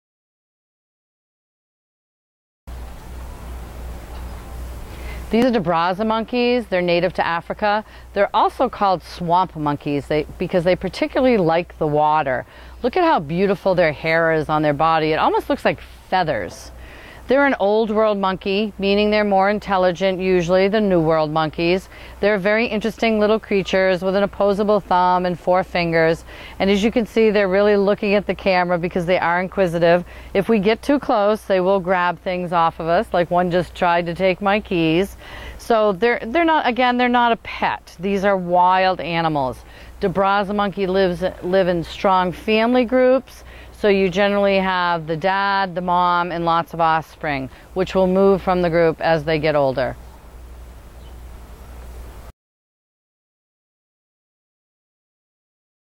Debrazza's Monkeys with Betsey at Southwick's Zoo in Mendon MA
Learn about the Debrazza's Monkeys that live at Southwick's Zoo in Mendon, MA. To see our full exhibit list, visit our